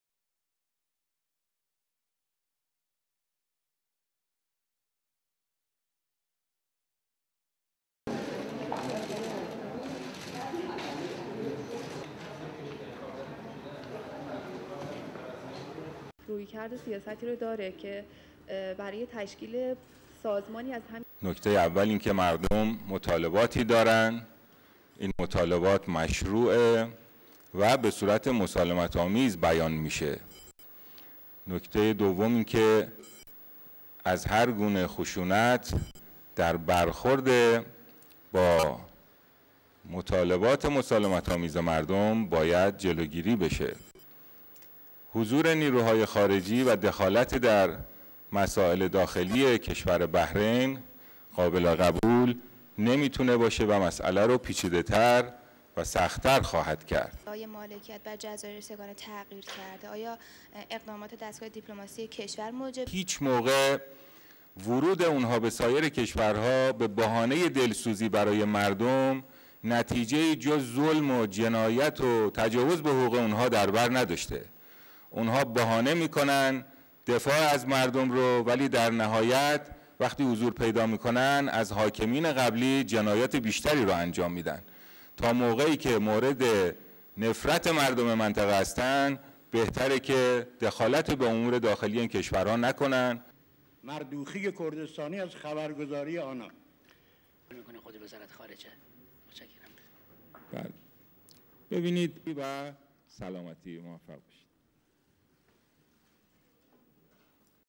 Iranian FM spokesman on Bahrain, Libya
NO ACCESS BBC PERSIAN / NO ACCESS VOA PERSIAN
1. Low angle of Iranian Foreign Ministry spokesman, Ramin Mehmanparast approaching podium
2. Close-up of hand taking notes
3. Wide of Mehmanparast speaking at podium
4. SOUNDBITE (Farsi) Ramin Mehmanparast, Iranian Foreign Ministry Spokesman:
"The first point is that people (Bahraini protesters) have demands which are legitimate and are uttered peacefully. The second point is that any violent confrontation with peaceful demands of people must be avoided and presence of foreign forces and their interference in the internal affairs of Bahrain is unacceptable and just makes the issue more complicated and difficult."
5. Pan of the presser
6. SOUNDBITE (Farsi) Ramin Mehmanparast, Iranian Foreign Ministry Spokesman: (Reacting to approval of a no-fly zone in Libya)
"Their interference in other countries with the pretext of sympathising with people of those countries have yielded no result but more oppression, violation and crimes against those people. They use it (defending rights of those people) as pretext but after they entered (those countries) they commit crimes even more than the previous rulers of those countries. They should not interfere in the internal affairs of these countries as long as they are hated by people of the region."
7. Wide of presser
8. Close-up of photographer
9. Wide of Mehmanparast leaving the podium
STORYLINE
Iran's Foreign Ministry spokesman has denounced the deployment of a Saudi-led military force to Bahrain to prop up the monarchy in the tiny island nation against widening demonstrations by the Shiite-led opposition.
Ramin Mehmanparast said on Tuesday that the "presence of foreign forces in Bahrain is unacceptable" and would only complicate the crisis in the Sunni-led kingdom.
The statement is the strongest reaction from Shiite powerhouse Iran on Monday's deployment of about 1,000 troops from Saudi Arabia and other members of the Gulf Cooperation Council.
It was the first cross-border military operation to quell unrest since the Arab world's rebellions began in December.
Bahrain is a key US ally and host of the US Navy's 5th Fleet. Its government is increasingly at odds on how to quell escalating opposition demonstrations, inspired by the wave of protests gripping the Arab world since December.
Opposition groups immediately denounced the intervention as an occupation that pushed the kingdom dangerously close to a state of "undeclared war."
Bahrain's majority Shiite Muslims see an opportunity to rid themselves of two centuries of rule by a Sunni monarchy. But Gulf Sunni leaders worry that any cracks in Bahrain's ruling system could threaten their own foundations. Protests are already flaring in Oman, Kuwait and even tightly ruled Saudi Arabia.
Gulf leaders are also concerned that political gains by Bahrain's Shiites might give Shiite Iran a stepping stone to its arch-rival Saudi Arabia, connected to Bahrain by a wide causeway.
The worries about Iran have persisted, even though there are no apparent links between Tehran and Bahrain's opposition.